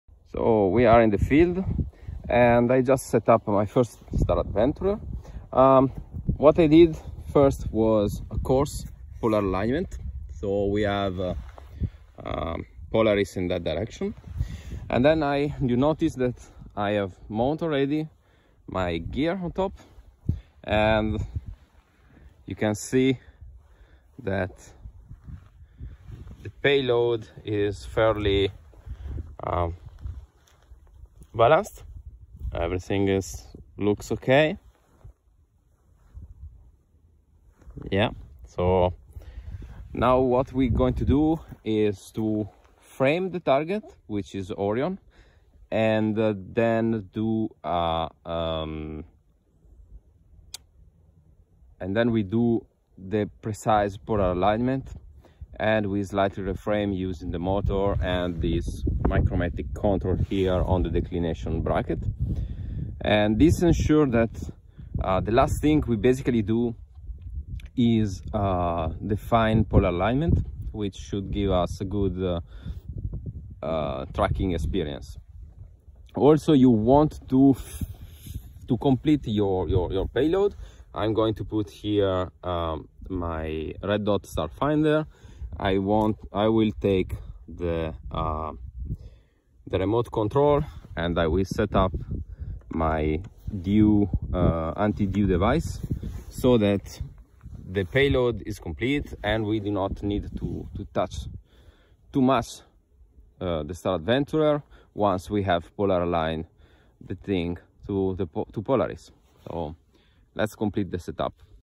Setting Up The Star Adventurer
In this short video I show you how I set up the Star Adventurer when under the stars.